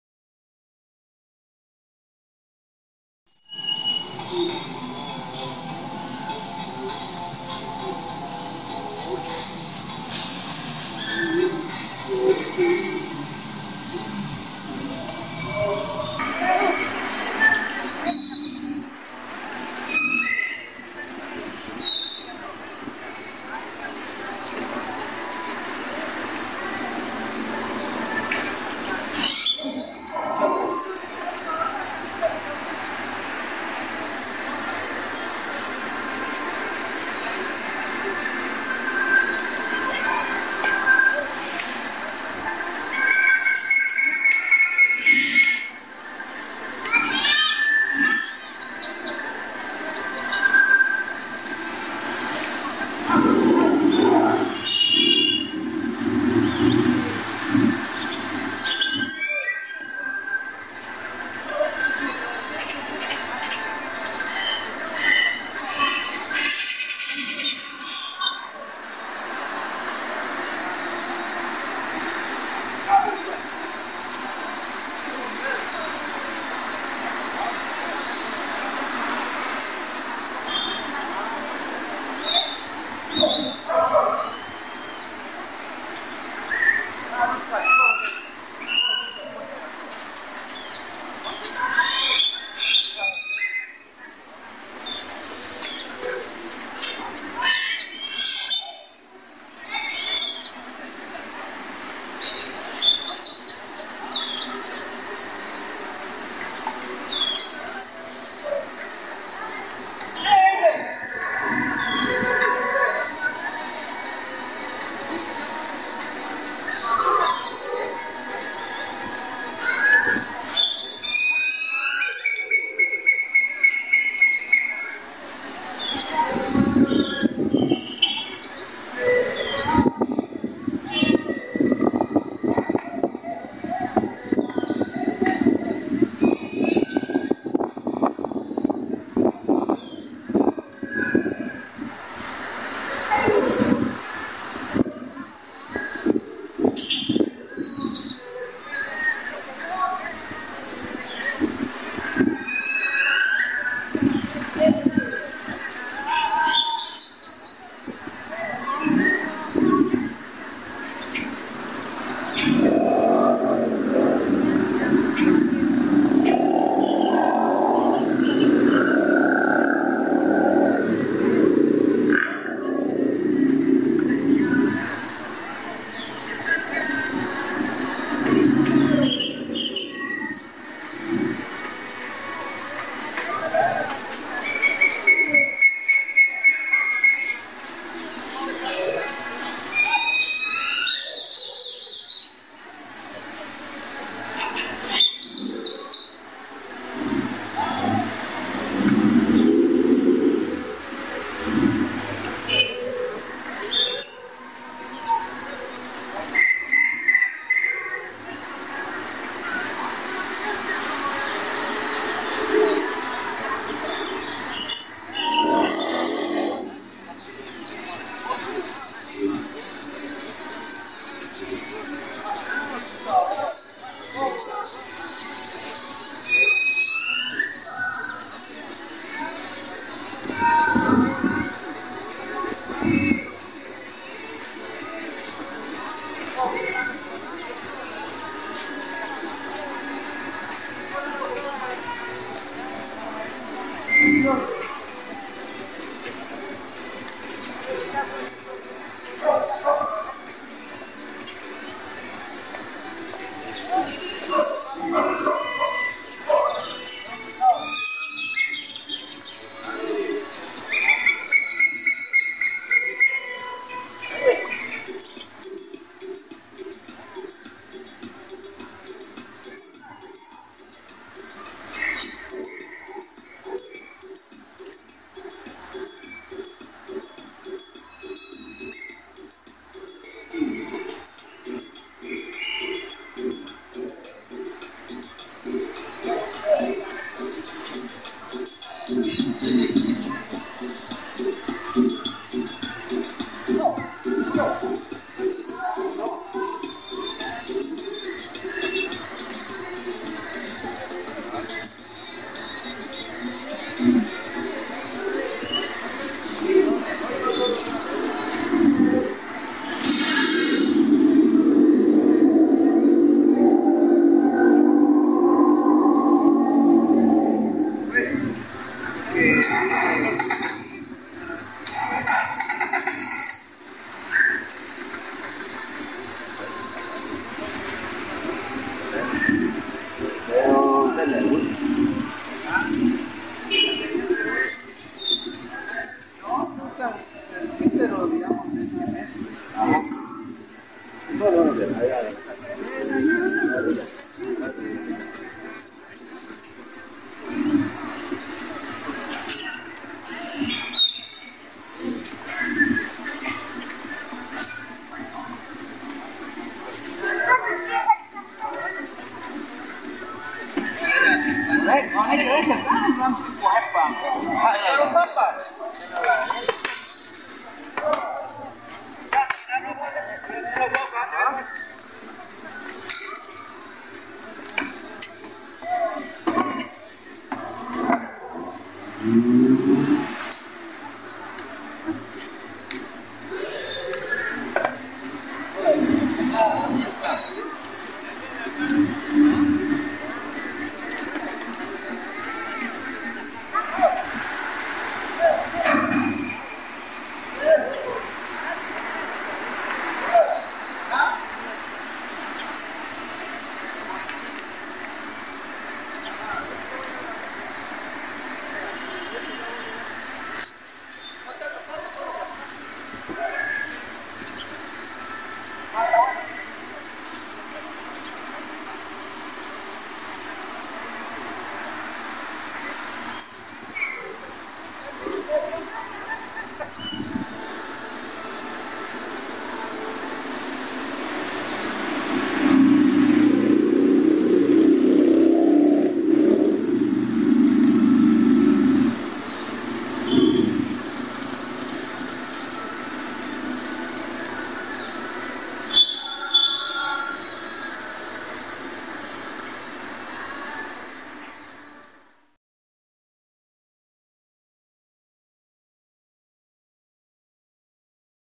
Parque Central "La Libertad" De San Cristóbal, Alta Verapaz, Guatemala. (30/Octub/23) 🌎 🇬🇹 📱🚶
Audio: C.N.E.C.N.)

Grabaciones En El Mes De Octubre Del Año Presente 2023. En El Departamento De Alta Verapaz, GT. 🇬🇹 🚶📱